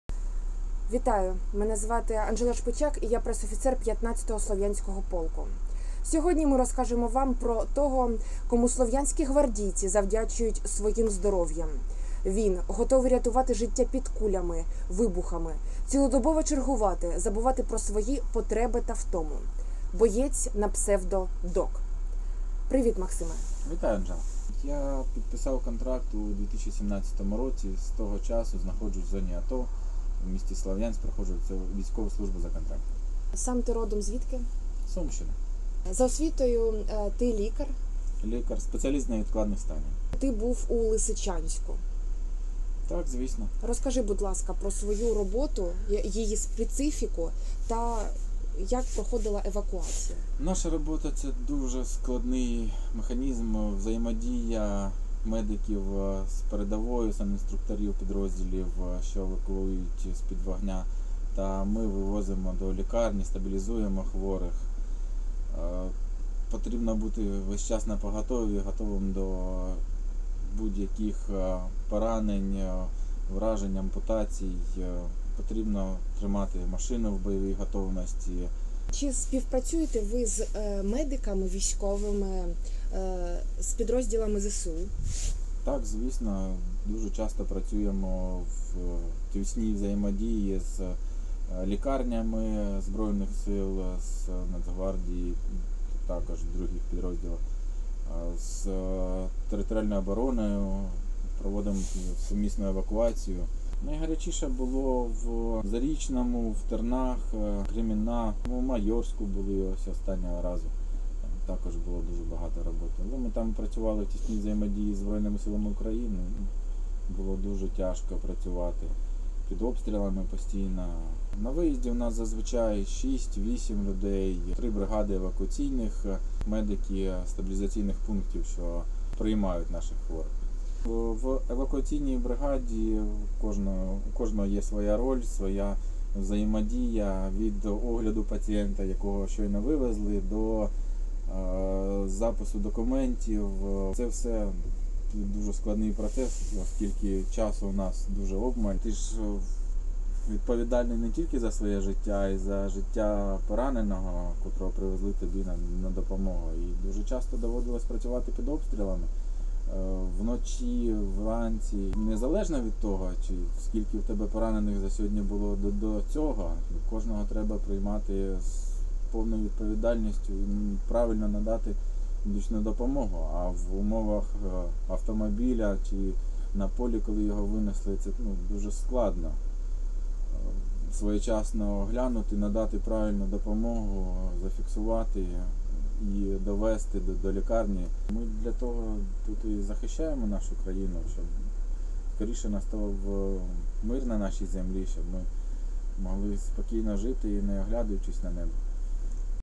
"Я - медик, і я вважаю, що треба допомагати країні. Якщо не ми, то хто, крім нас?"
Док – бойовий медик, капітан медичної служби. В армії він понад 5 років. За його плечима сотні виїздів за пораненими, евакуація з передової та надання домедичної допомоги. Нерідко все це під обстрілами.

«Я – медик, і я вважаю, що треба допомагати країні. Якщо не ми, то хто, крім нас? Медична допомога, яку зараз ми надаємо бійцям, це дуже важливо, тому що це – життя, врятовані життя», – говорить Док. Він був у цілому ряді найгарячіших точок Донецької та Луганської областей – в Лисичанську, Зарічному, Тернах, Кремінній.

Медики бачать багато болю та крові, але вони намагаються триматися і підтримувати бойових побратимів.